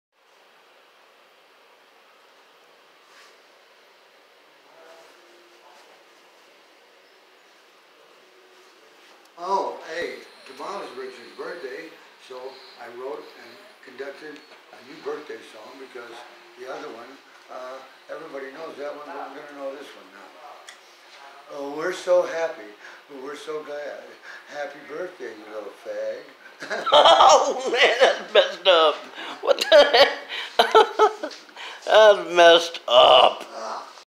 Gay Birthday Song!!!
Meet my good friend, Frank, everybody. He's just joking around with me, because it was my 49th Birthday today.